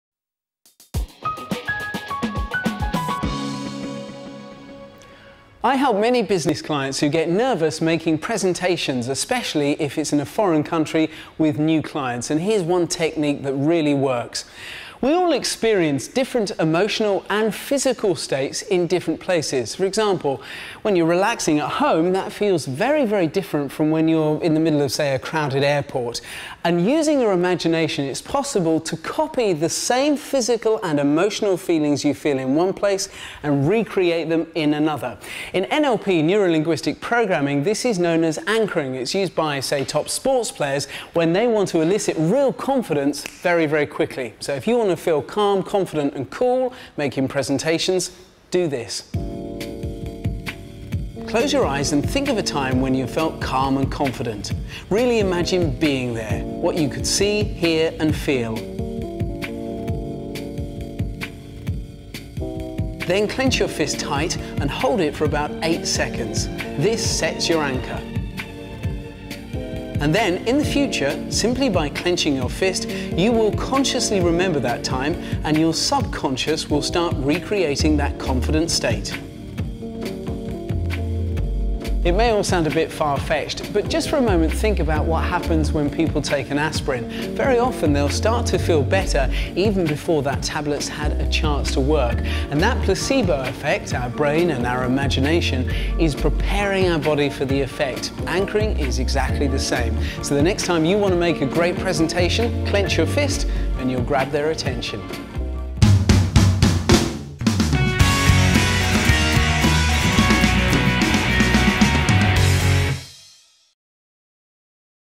Become calmer during business presentations
A quick, subtle way to improve your confidence during business presentations and meetings